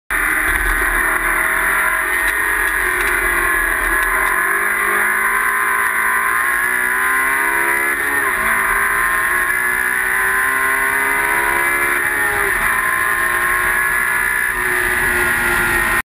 Real GT Ariel Atom at Rockingham
You can get your Ariel Atom fix with The Real Gran Turismo! Enjoy 15 seconds of Atom awesomeness. Get a full hour on track with tuition from £600.